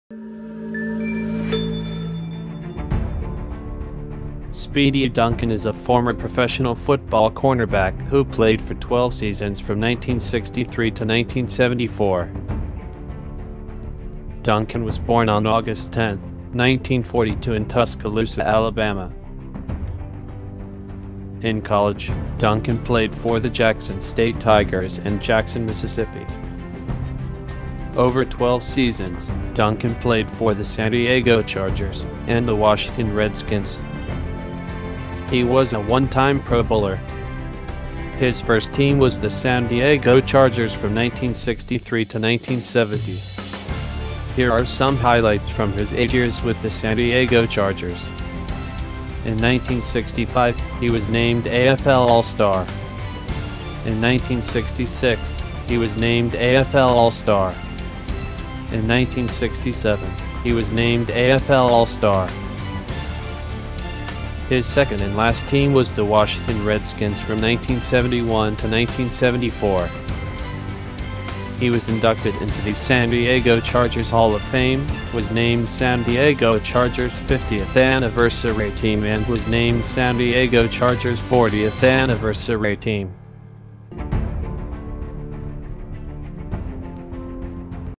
Speedy Duncan: Football Cornerback
Speedy Duncan is a former professional football cornerback who played for 12 seasons from 1963 to 1974.

In college, Duncan played for the Jackson State Tigers in Jackson Mississippi.

Professionally, Duncan played for the San Diego Chargers, and the Washington Redskins.

In 1965, he was named A.F.L. All-Star.
In 1966, he was named A.F.L. All-Star.
In 1967, he was named A.F.L. All-Star.

He was inducted into the San Diego Chargers Hall of Fame, was named San Diego Chargers 50th Anniversary Team and was named San Diego Chargers 40th Anniversary Team.